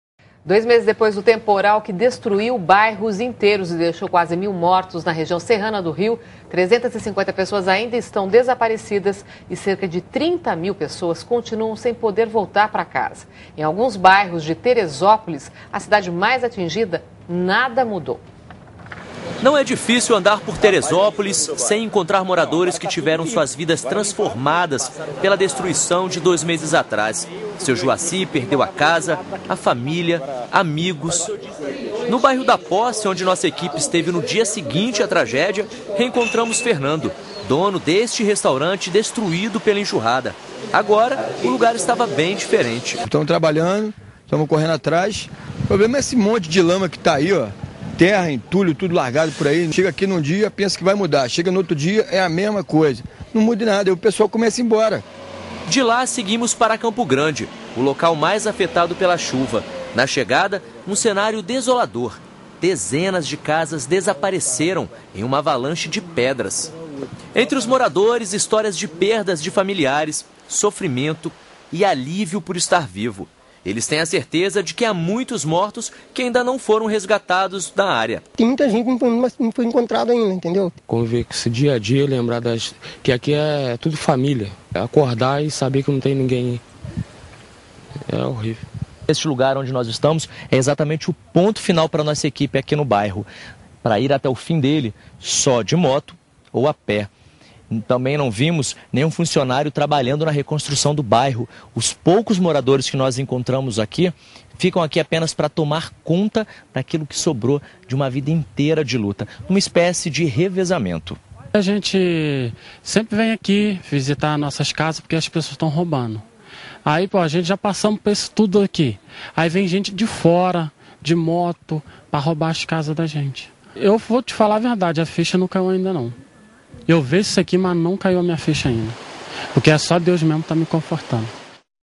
Chuvas RJ
População Serrana do Rio ainda sofre com as consequências das chuvas.